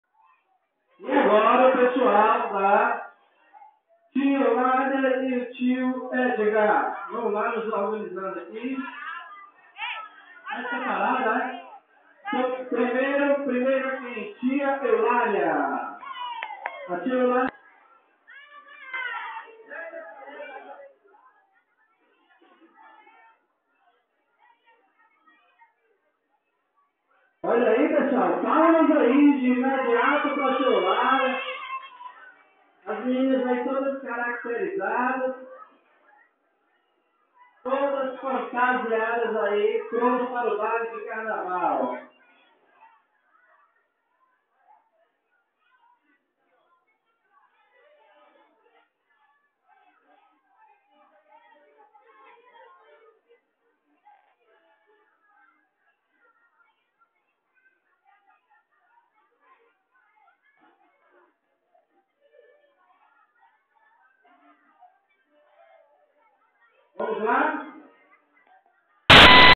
Carnaval-Rachel de Queiroz 4.Ley
A Diretoria da Escola Rachel de Queiroz, promovendo o carnaval dos seus alunos.
O Carnaval de Rachel de Queiroz foi festejado nessa manhã de sexta feira (17 de Fevereiro de 2012). A garotada fizeram uma grande algazarra quando saíram da Escola. Muita alegria e sujeira na frente da escola. O que os organizadores não previa era a alegria contagiante que envolveu essa cena. As crianças proibidas pela a diretoria de sujar a escola e promover o autentico carnaval. Impuseram a eles, as crianças a fazerem uma espécie de conscientização ao meio ambiente. Economizar energia, água.. blá blá... Pena que estamos fazendo do carnaval um chega pra lá, por parte de pessoas estúpidas.